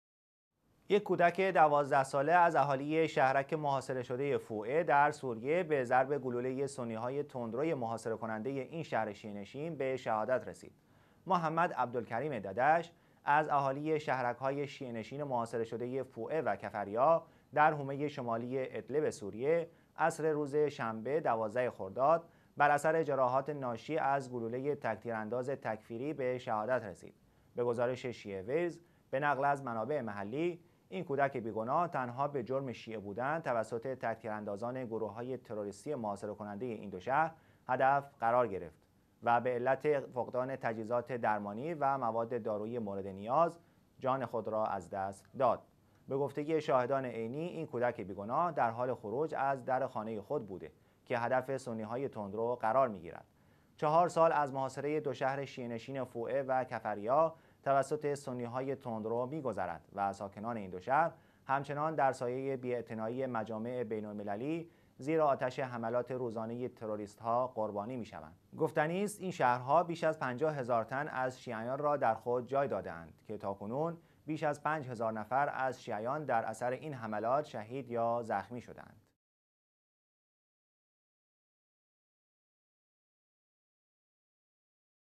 شهادت يک کودک شيعه، در شهر« فوعه» سوريه
کودک ١٢ ساله از اهالی شهرک محاصره شده فوعه در سوریه توسط سنی‌های تندروی محاصره کننده این شهر شیعه نشین قربانی شد.

«محمد عبد الکریم ددش» دوازده ساله، از اهالی شهرک‌های شیعه نشین محاصره شده فوعه و کفریا، در حومه شمالی ادلب سوریه، عصر دیروز (شنبه 12 خرداد)، بر اثر جراحات ناشی از گلوله تک تیراندازان تکفیری، به شهادت رسید.

به گزارش شیعه ویوز به نقل از منابع محلی، این کودک بی گناه تنها به جرم شیعه بودن، توسط تک تیراندازان گروه‌های تروریستی محاصره کننده این دو شهر، هدف قرار گرفته و بدلیل نبود تجهیزات درمانی و مواد دارویی مورد نیاز، جان می‌دهد.

به گفته شاهدان عینی این کودک بی گناه در حال خروج از درب خانه خود بوده که هدف سنی های تندرو قرار می گیرد.

چهار سال از محاصره دو شهر شیعه نشین فوعه و کفریا در استان ادلب سوریه توسط سنی‌های تندور می‌گذرد و ساکنان این دو شهر همچنان در سایه بی اعتنایی مجامع بین المللی، زیر آتش حملات روزانه تروریست ها قربانی می‌شوند.

گفتنی است این شهرها بیش از 50 هزار تن از شیعیان را در خود جای داده اند که تاکنون بیش از پنج هزار نفر از شیعیان در اثر این حملات شهید یا زخمی شده‌اند.